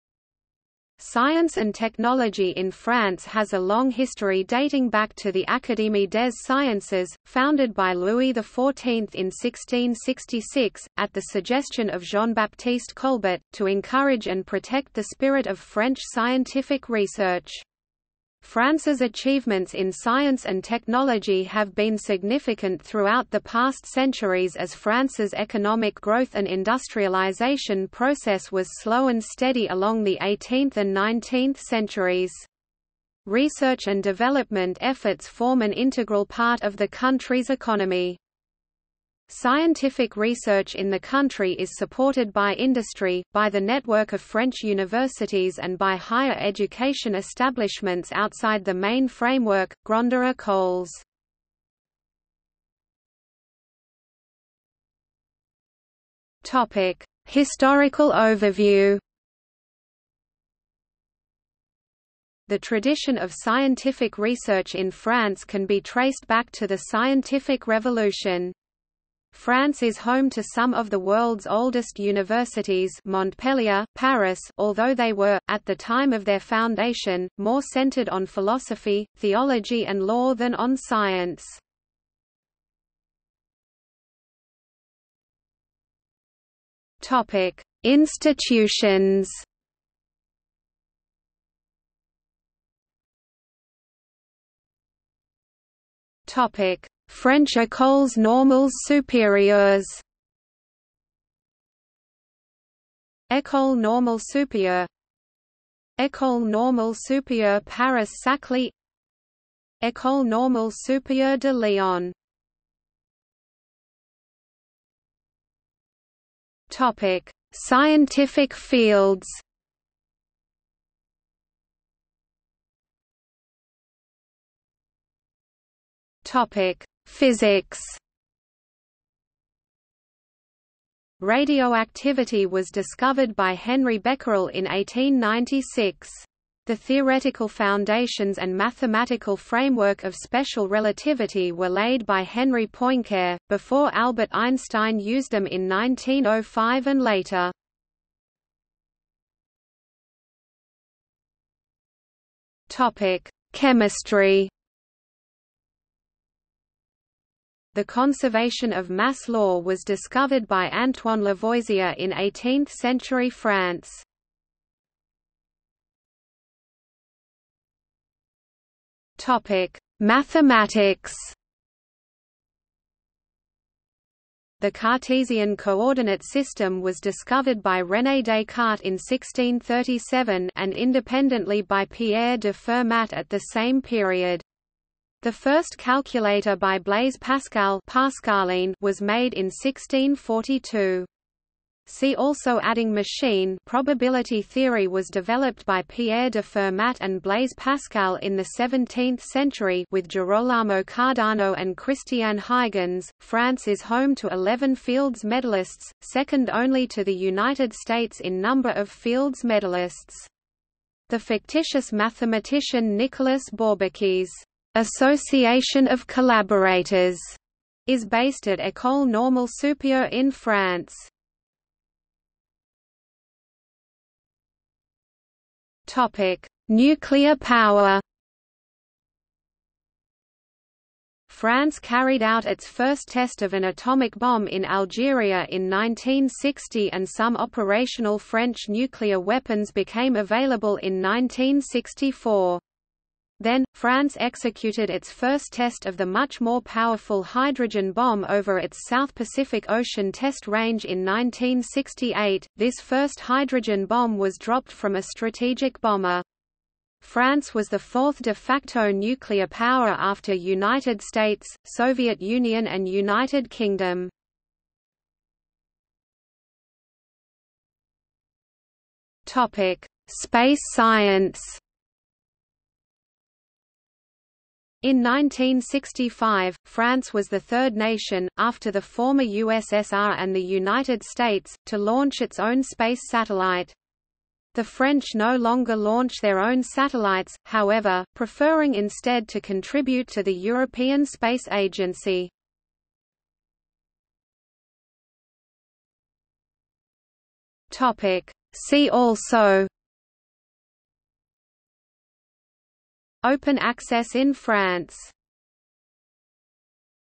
Science and technology in France | Wikipedia audio article
00:00:52 1 Historical overview
00:01:23 2 Institutions
00:01:33 2.1 French écoles normales supérieures
00:01:53 3 Scientific fields
00:02:03 3.1 Physics
00:02:29 3.2 Chemistry
00:02:45 3.3 Mathematics
00:03:42 3.4 Nuclear power
00:04:26 3.5 Space science
00:04:54 4 See also

- Socrates

SUMMARY
Science and technology in France has a long history dating back to the Acádémie des Sciences, founded by Louis XIV in 1666, at the suggestion of Jean-Baptiste Colbert, to encourage and protect the spirit of French scientific research. France's achievements in science and technology have been significant throughout the past centuries as France's economic growth and industrialisation process was slow and steady along the 18th and 19th centuries. Research and development efforts form an integral part of the country's economy.
Scientific research in the country is supported by industry, by the network of French universities and by higher education establishments outside the main framework, Grandes écoles.